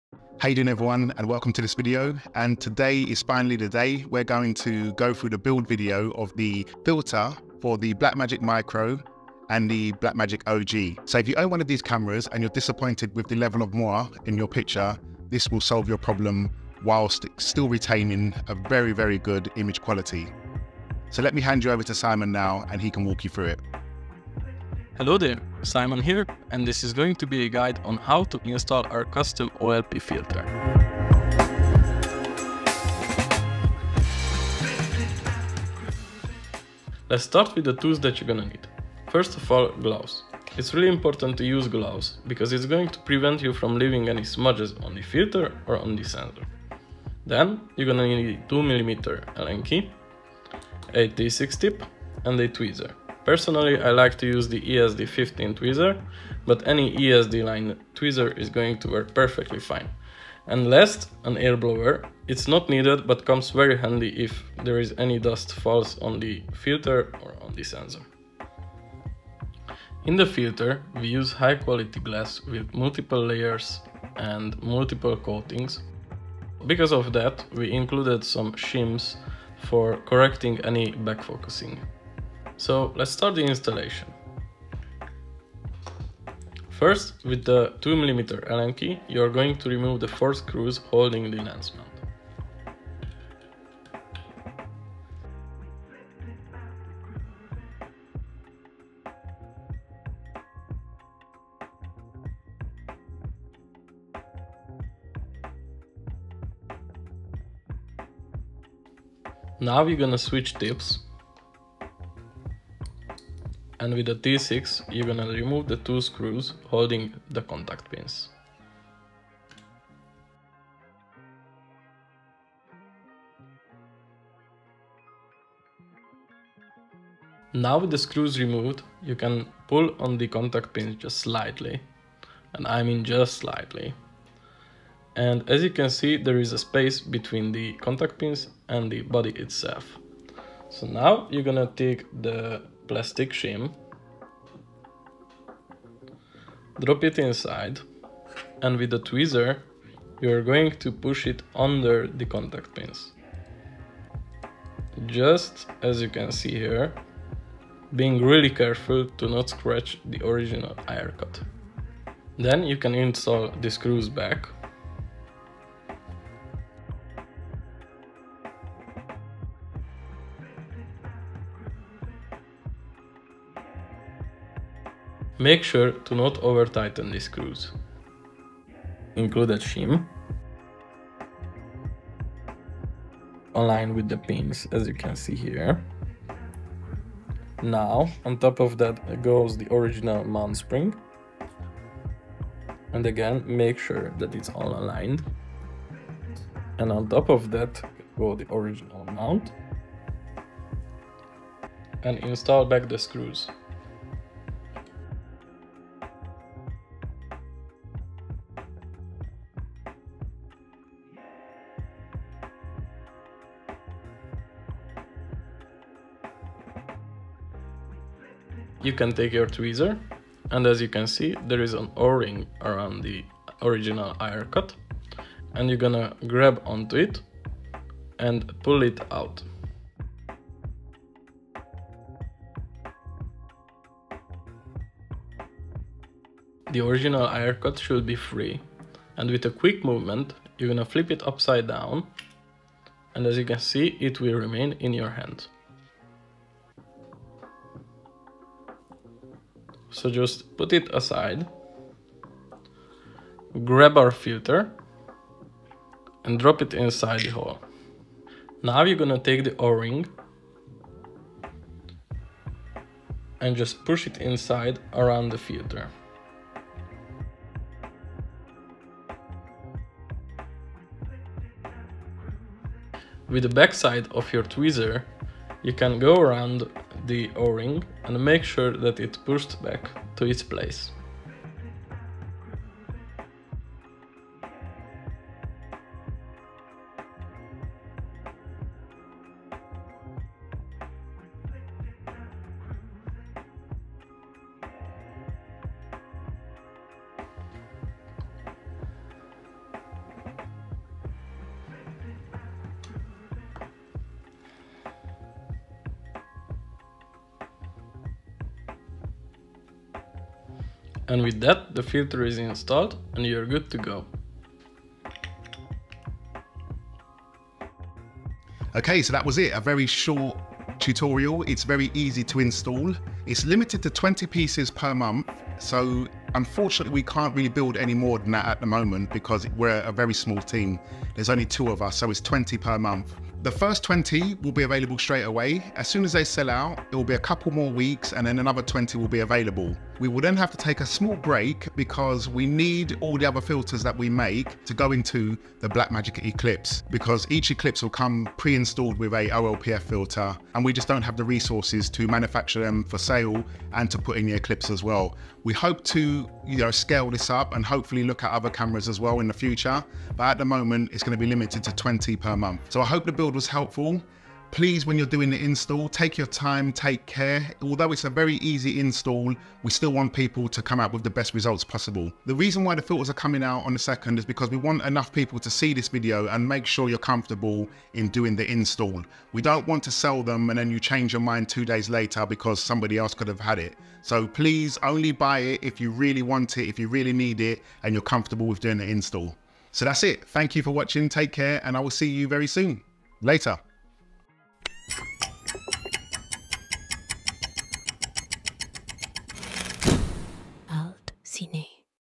BLACKMAGIC OLPF - Installation Guide UPDATE
UPDATE video with clearer instructions.

Here is the build video for the BMPCC OG & BMMCC OG cinema cameras.
The filters will be available on the 02/05/2024 thats the 2nd May for my American family.

The filter work with 0.71x speedbooster. I can't guarantee it'll work with the others

Only 20 available and another 20 soon after.

BLACKMAGIC OLPF - Installation Guide